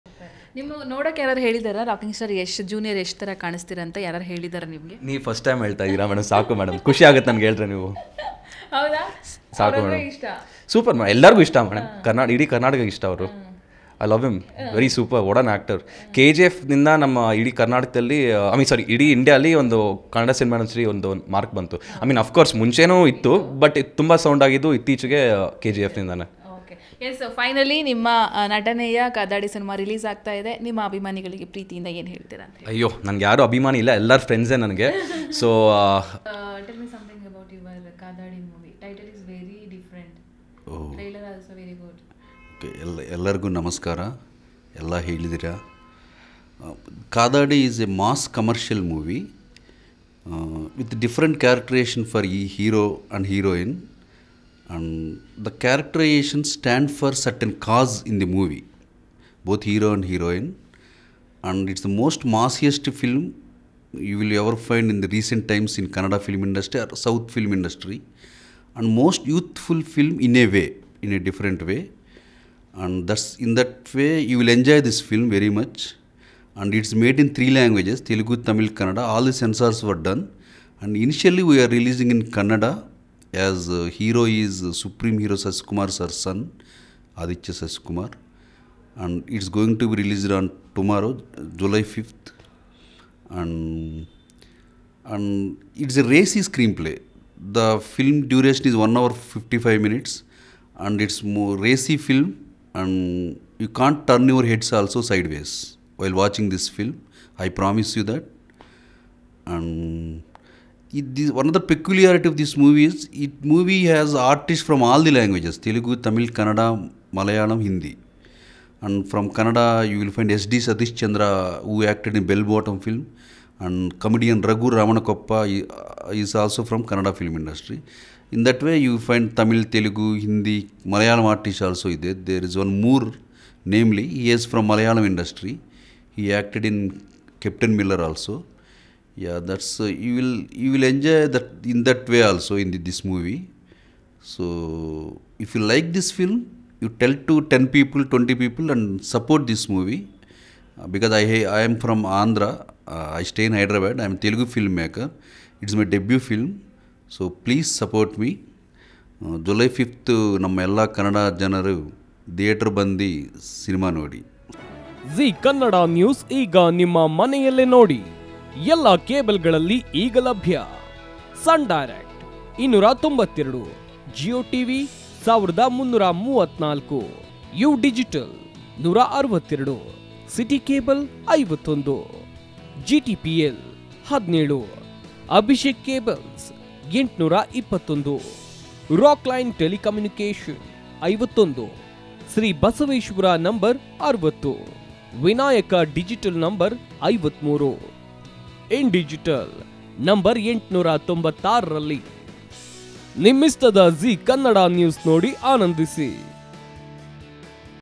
Aditya Shashikumar Interview | Kadadi Kannada Film | ರಾಕಿಂಗ್‌ ಸ್ಟಾರ್‌ ಯಶ್‌ ಅಂದ್ರೆ ಎಲ್ಲರಿಗೂ ಇಷ್ಟ
ರಾಕಿಂಗ್‌ ಸ್ಟಾರ್‌ ಯಶ್‌ ಅಂದ್ರೆ ಎಲ್ಲರಿಗೂ ಇಷ್ಟ
ಕಾದಾಡಿ ಸಿನಿಮಾ ನೋಡಿ ಸಪೋರ್ಟ್‌ಮಾಡಿ